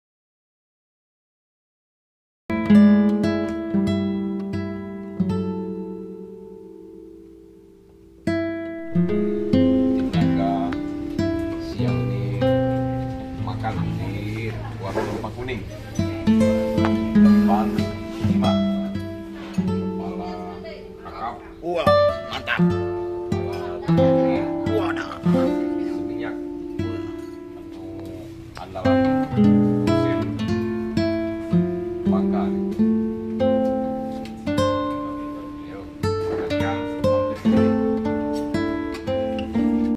Berburu Kuliner Khas Bangka
Usai Melaksanakan Tugas Tim Buser Naga Cicipi Lempah Kuning Khas Bangka Di Depan PT  Timah @Marizaanto45